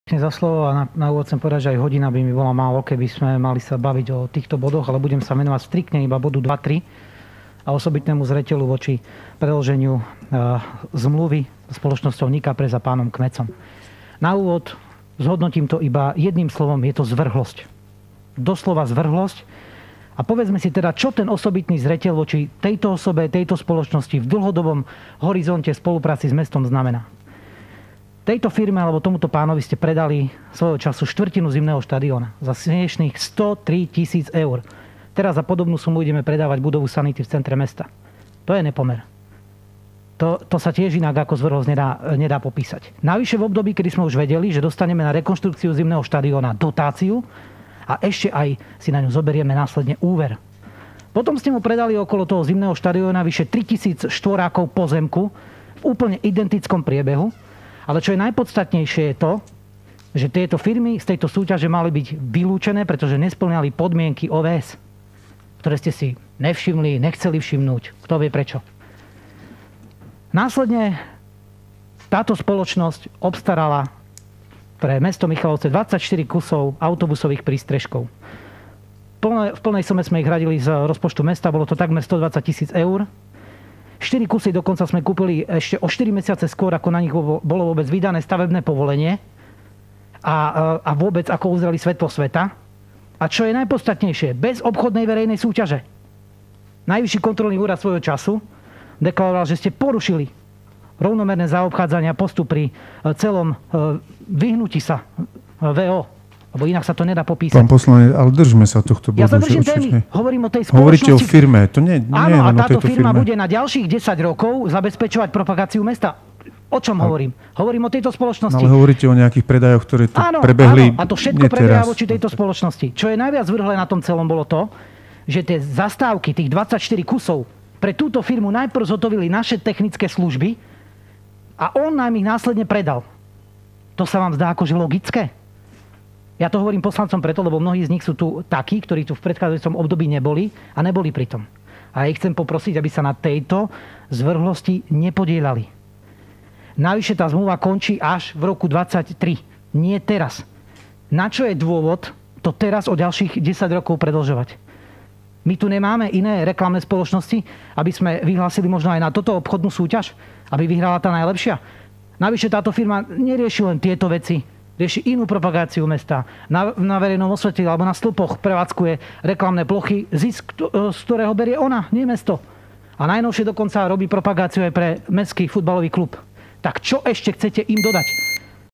Protekcia tzv. našich ľudí
10 ročná zmluva na propagáciu a prezentáciu mesta už na druhé desaťročie bez verejnej súťaže, odpredaná 1/4 zimného štadióna aj s priľahlým pozemkom, odkup autobusových zastávok bez verejnej súťaže, ktoré predtým vyrobila mestská organizácia, reklamné plochy na mestských stĺpoch verejného osvetlenia za ktoré inkasuje peniaze súkromná firma, marketing mestského futbalového klubu a mnohé iné... Toto všetko v réžii jednej firmy a rodiny, monopol a protekcia, ktorá nemá obdoby...